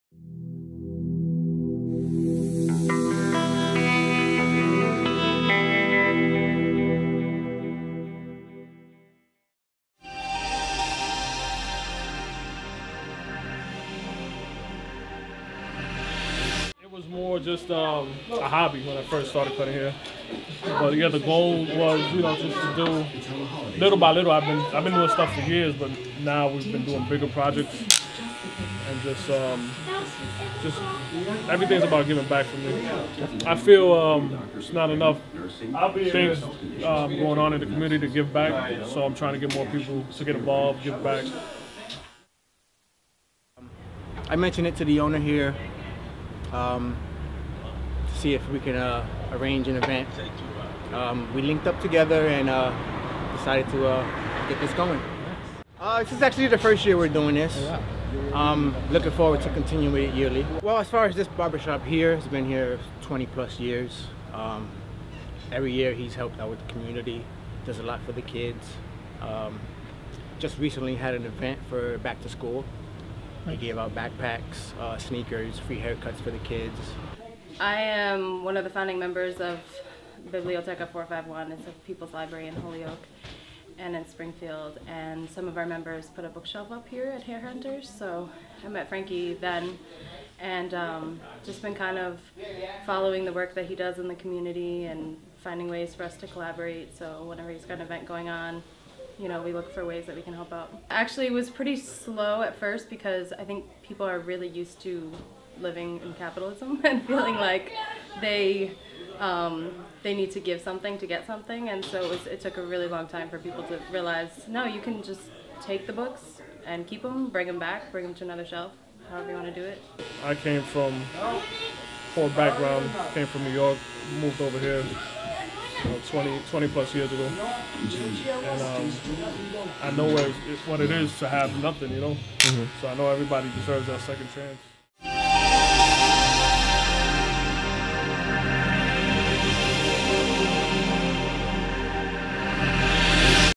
Haircuts in Holyoke - Hair Hunters Nov. 22, 2015
On Nov. 22, Hair Hunters barbershop in Holyoke, under the caring and watchful eye of owner Frankie Cardona, offered free haircuts and free coats to those less fortunate. This was Hair Hunters' second year offering such an event. Video by Conor Snell.